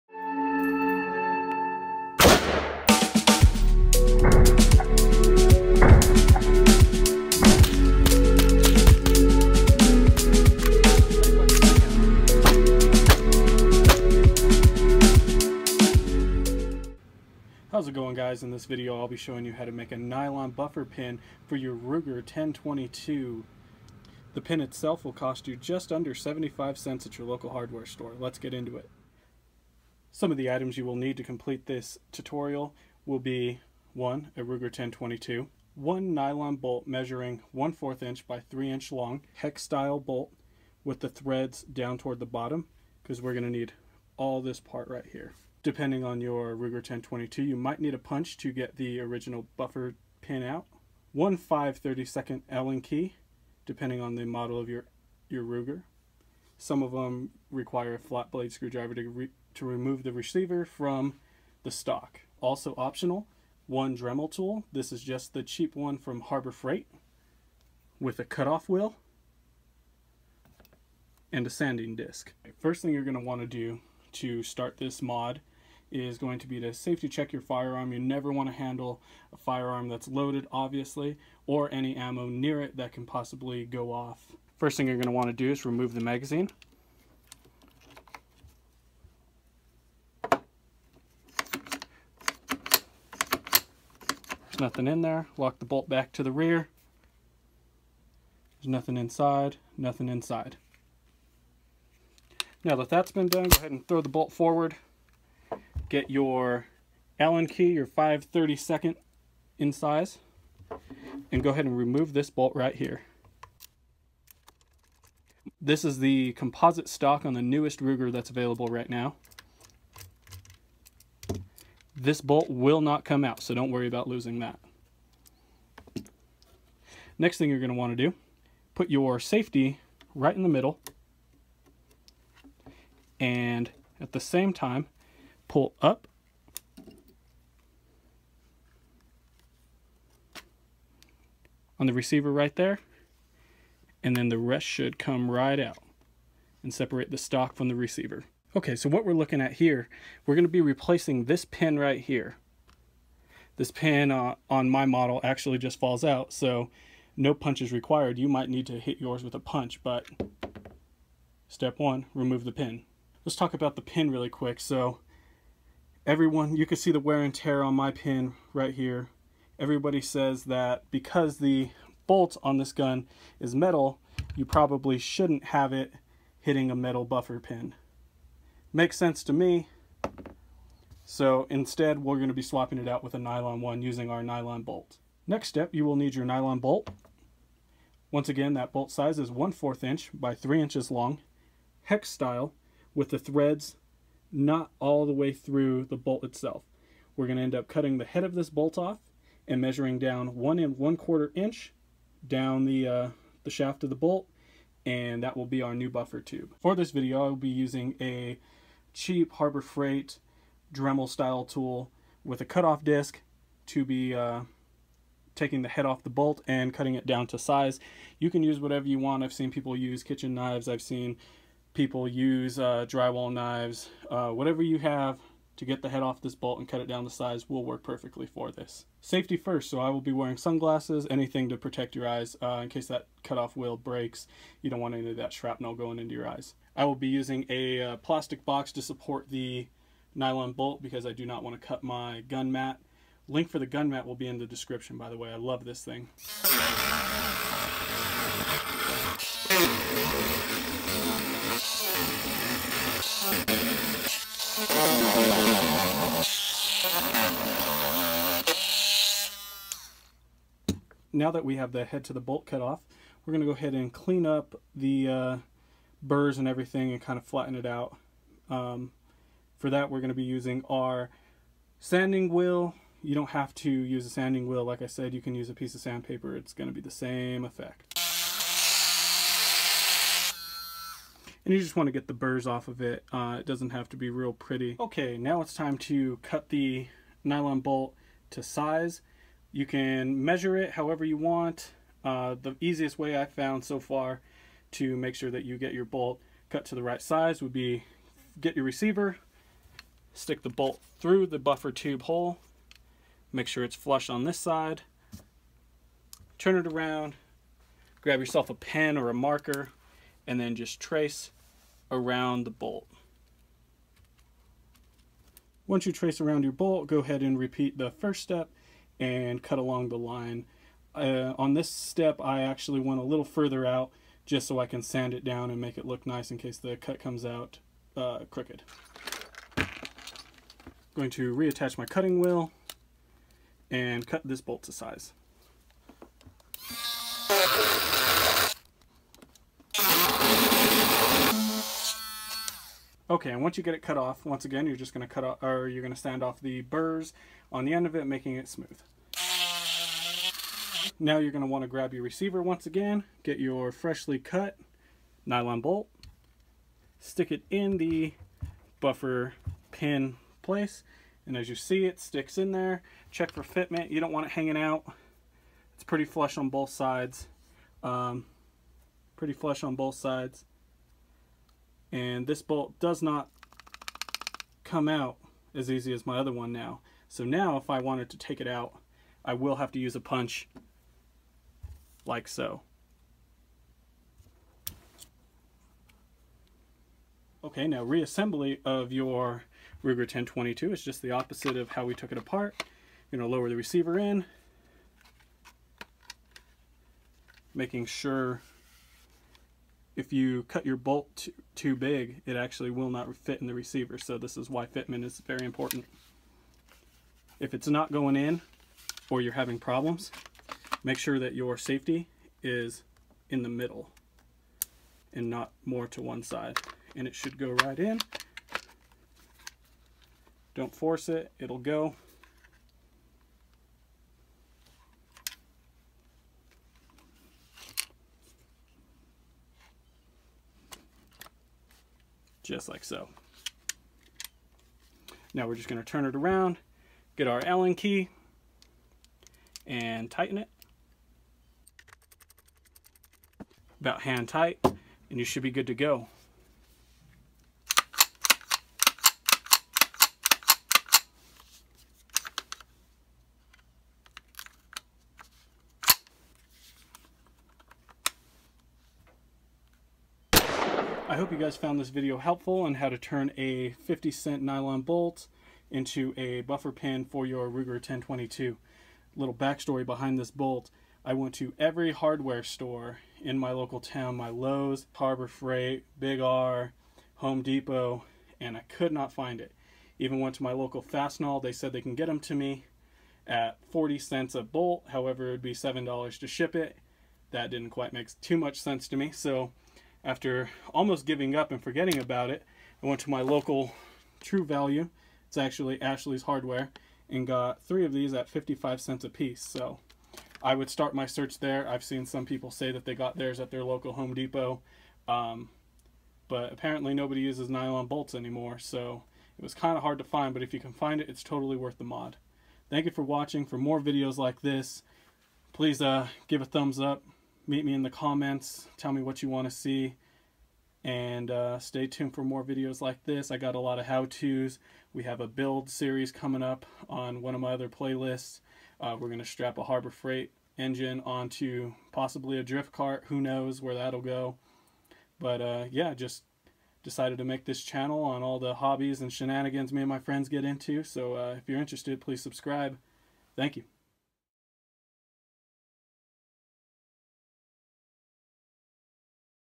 How To Make Ruger 1022 Buffer Pin
In this video I will show you how to make a nylon buffer pin for your Ruger 10/22 out of a .55 cent nylon bolt!

Nylon bolt size: 3in X 1/4